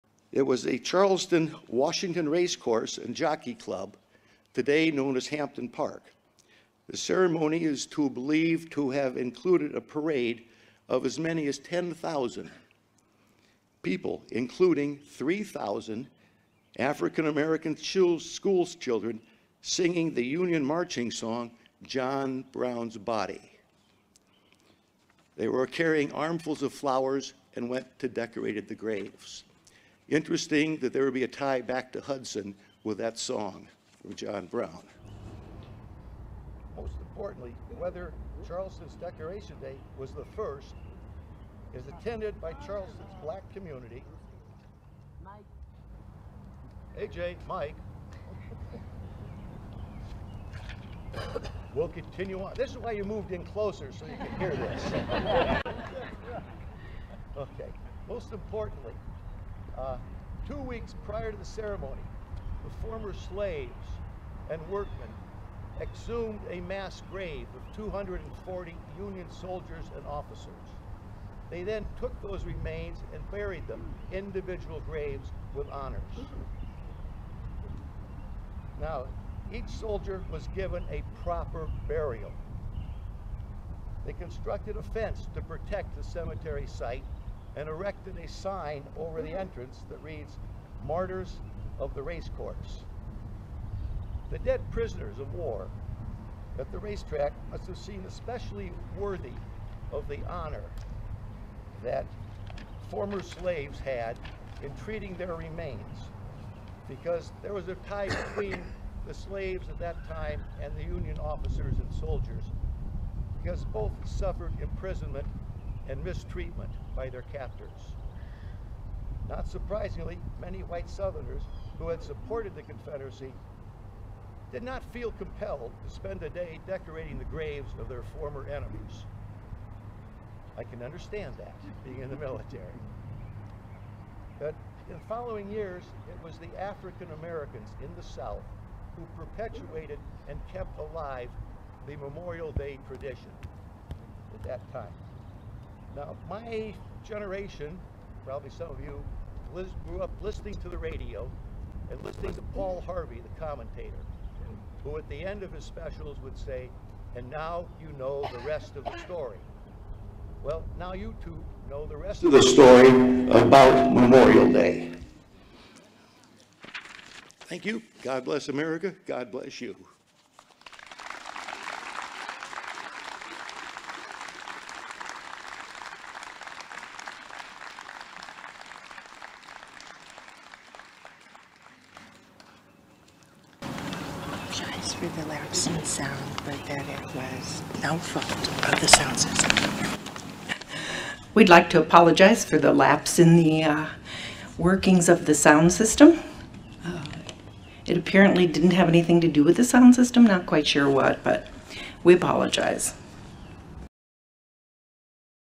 Audio cut during veteran's speech on Black people’s role in Memorial Day at Hudson ceremony
It was no audio glitch.  It was an audio cut during a veteran's speech.

It was a normal Memorial Day observance in Hudson on Monday. Until it wasn't.

Organizers of a Memorial Day ceremony turned off a speaker’s microphone when the former U.S. Army officer began talking about how freed Black slaves had honored fallen soldiers soon after the Civil War.

Retired Army Lt. Col. Barnard Kemter tells 3News he thought it was an audio glitch at first, but it was no glitch at all. His microphone was purposely muted for several minutes.

Kemter said he included the story in his speech because he wanted to share the history of how Memorial Day originated. But organizers of the ceremony said that part of the speech was not relevant to the program’s theme of honoring the city’s veterans.

Cindy Suchan, chair of the Memorial Day parade committee and president of the Hudson American Legion Auxiliary, says it was either her or Jim Garrison, adjutant of the American Legion Post 464, who turned down the audio.

The Ohio American Legion is investigating the incident.